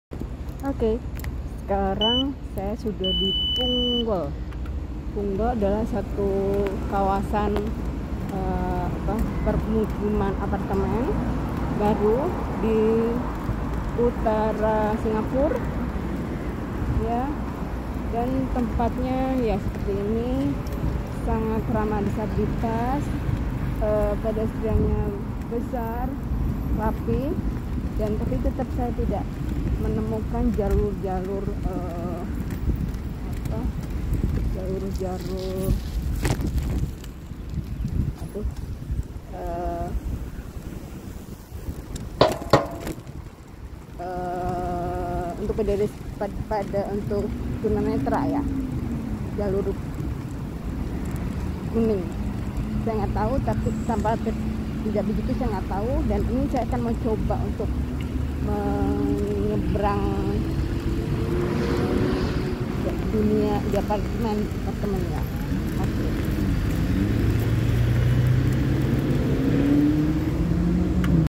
Kota Satelit Punggol ❤️ Bag.1
Kawasan Punggol dibentuk untuk sebuah kawasan peemukiman bagi warga Singapore,
Dibangun untuk sekitar 96.000 unit hunian, Punggol menjadi kota satelit yang sangat dicari di Singapore ❤️

Mulai direncanakan sejak taahunn 2005,
mulai dibangun sejak tahun 1996 dan terus berkembng dengan master-plan "Punggol 21" sampai "Punggol 21 Plus", sp tahun 2032 .